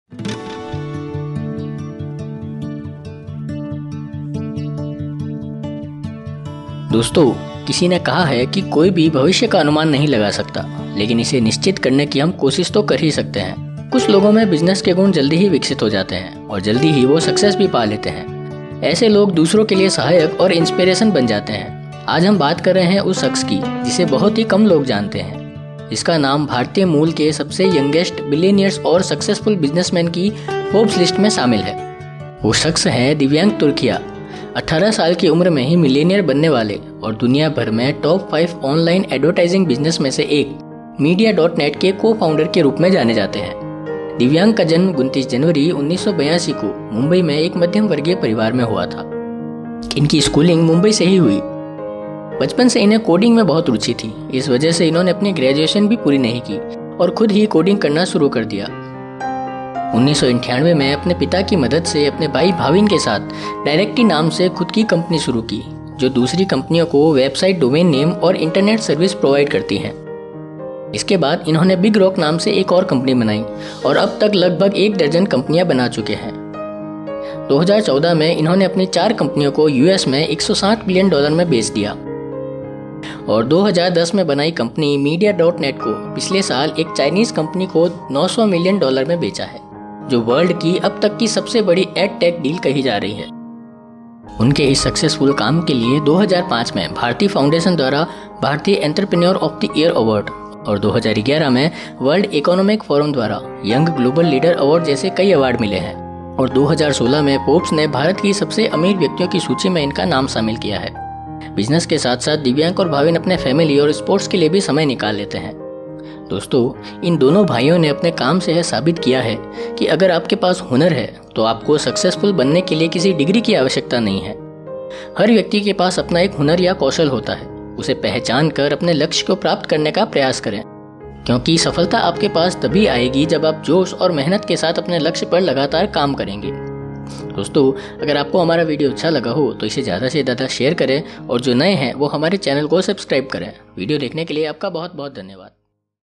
Divyank turakhia Entrepreneur Founder directi & Media.net brother of Bhavin Hindi Biography Networth
Divyank Turakhia is an Entrepreneur Founder directi and Media.net Indian-born computer-programmer, businessman, billionaire, serial entrepreneur, and investor. His net worth is estimated at $1.76 billion. He founded Media.net, which he sold in 2016 to a Chinese consortium for $900 million, the third-largest ever ad-tech deal.